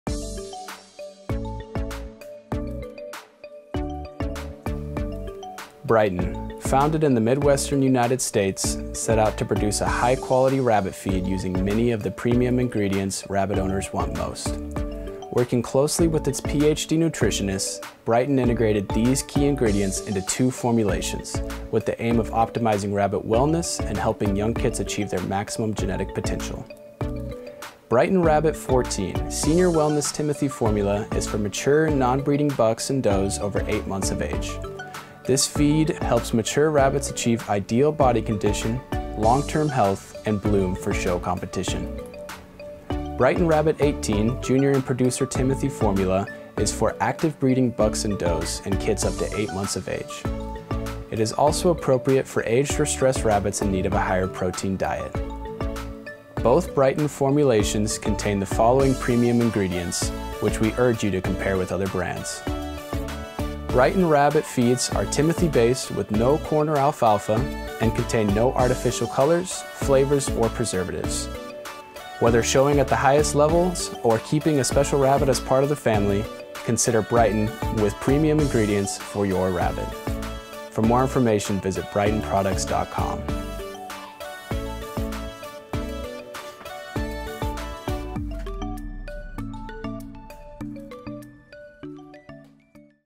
Brytin Rabbit 14 Senior Wellness Timothy Formula & Brytin Rabbit 18 Junior & Producer Timothy Feed
Brytin, founded in the Midwestern United States, set out to produce a high quality rabbit feed using many of the premium ingredients rabbit owners want most. Working closely with its PhD nutritionist, Brytin integrated these key ingredients into two formulations, with the aim of optimizing rabbit wellness and helping young kits achieve their maximum genetic potential.

Brytin Rabbit 14 Senior Wellness Timothy Formula is for mature non-breeding bucks and does over 8 months of age. This feed helps mature rabbits achieve ideal body condition, long term health, and ‘bloom’ for show competition.

Brytin Rabbit 18 Junior & Producer Timothy Formula is for active breeding bucks and does, and kits up to 8 months of age. It is also appropriate for aged or stressed rabbits in need of a higher protein diet.

Both Brytin formulations contain the following Premium Ingredients, (which we urge you to compare with other brands).

Brytin Rabbit feeds are Timothy based with no corn or alfalfa, and contain no artificial colors, flavors or preservatives. Whether showing at the highest levels, or keeping a special rabbit as part of the family, consider Brytin with Premium Ingredients for your rabbit.